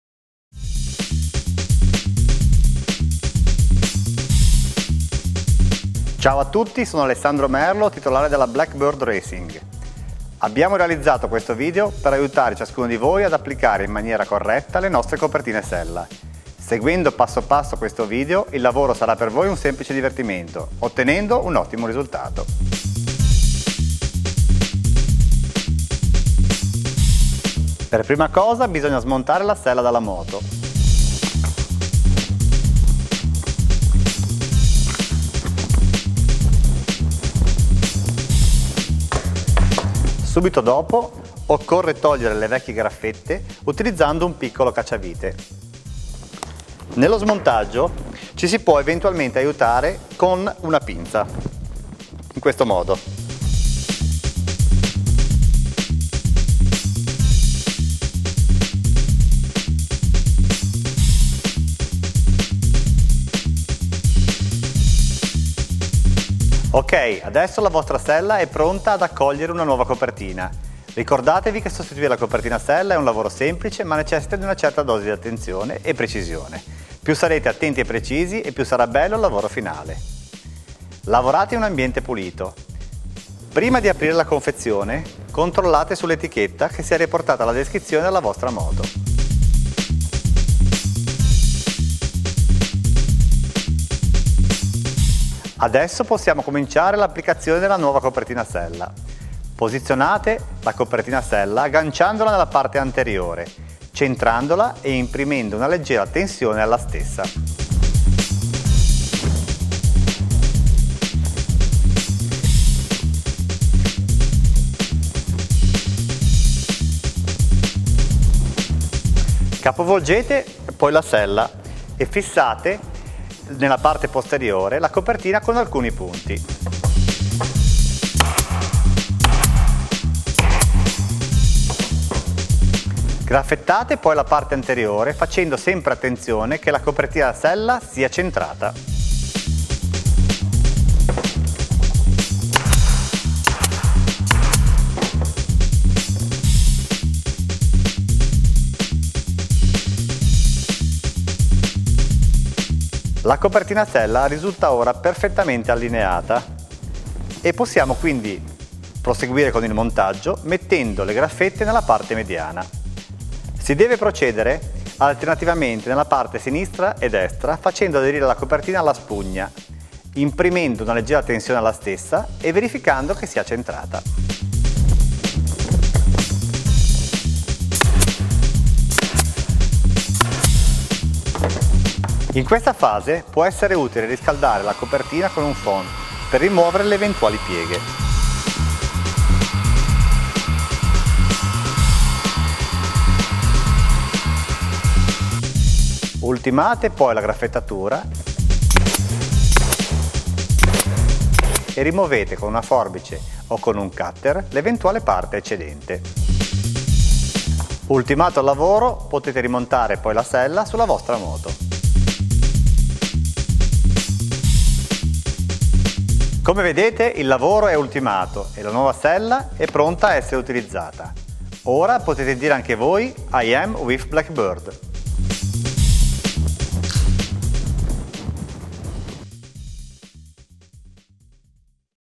Istruzioni per il montaggio delle copertine sella (Versione Italiana)
Questo video illustra il metodo corretto per il montaggio delle copertine sella Blackbird Racing, al fine di ottenere un risultato professionale.